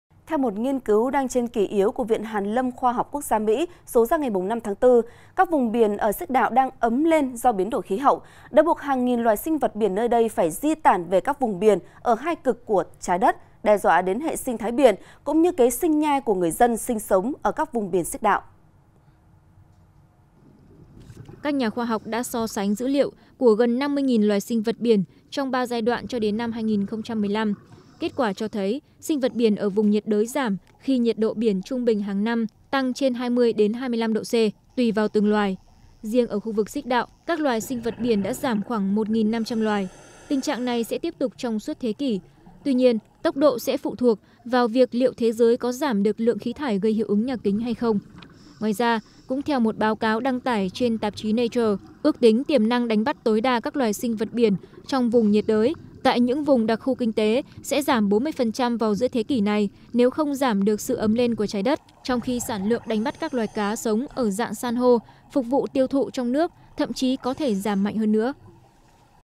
Gia tăng "di tản" của các loài sinh vật biển vùng xích đạo
Gia tăng "di tản" của các loài sinh vật biển vùng xích đạo | Truyền Hình Nhân Dân
1. Dự Báo Thời Tiết Hôm Nay